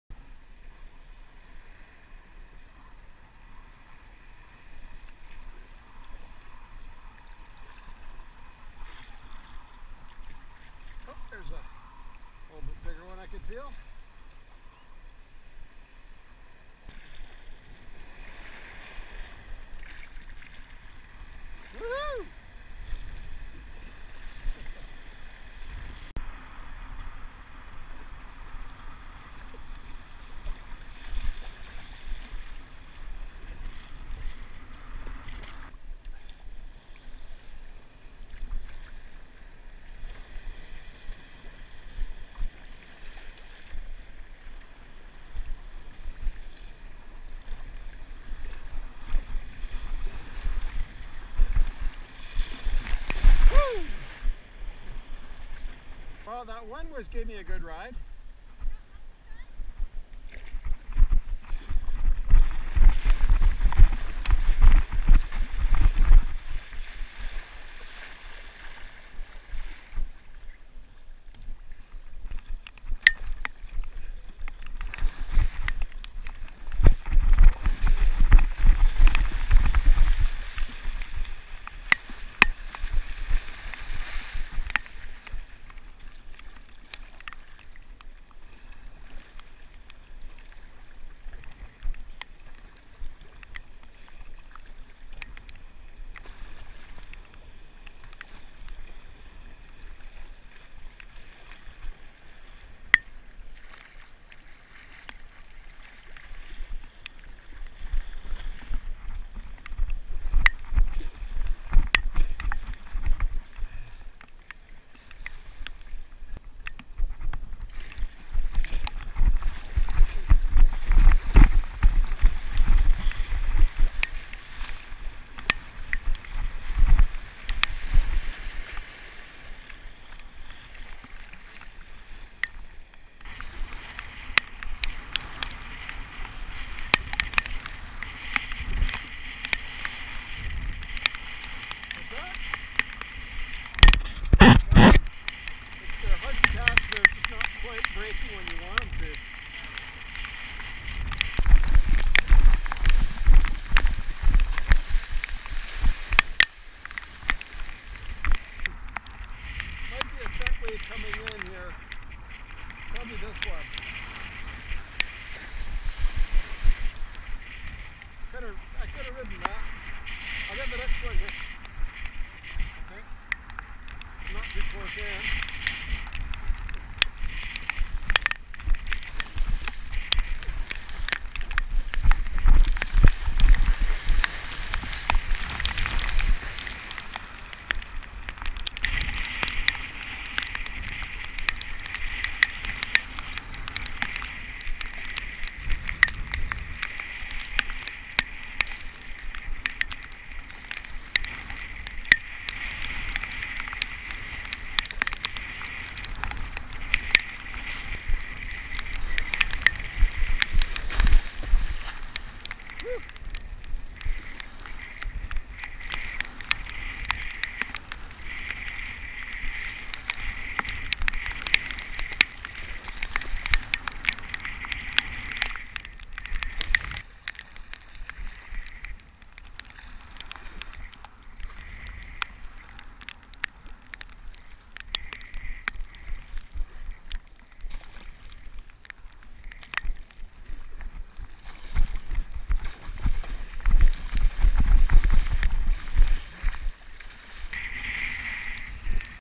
whifferJan18early
A day of frustrating mostly small waves that didn't break when you wanted them to.